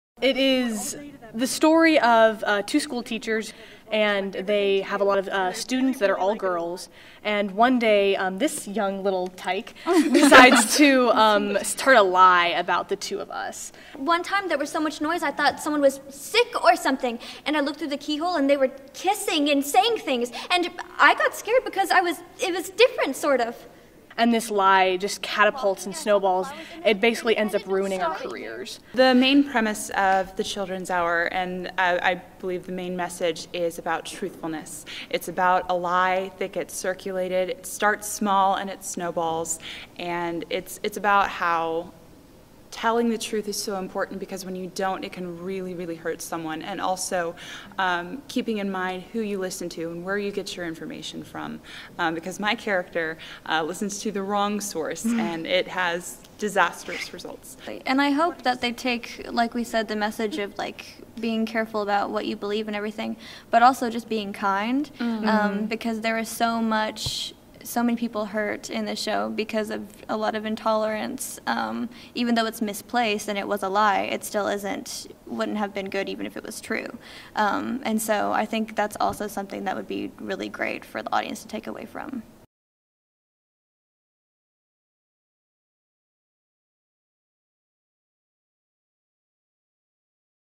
Groundbreaking play from the 1930s delivers powerful message for today's turbulent times
Columbus State University's  production of Lillian Hellman’s groundbreaking play “The Children’s Hour” Opens Nov. 2 at CSU’s Riverside Theatre Complex. The play made its debut in 1934 and details  how false accusations can spread and ruin lives.